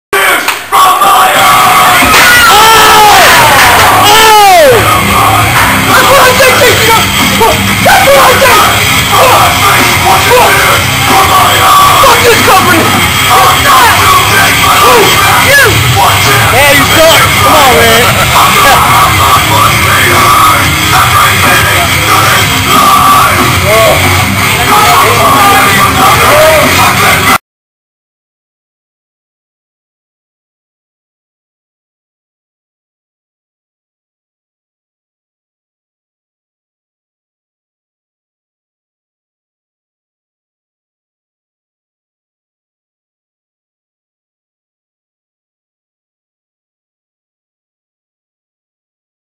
Chair shot 2 the dome
A chair shot that would make Joey Styles go OH MY GAWD!!!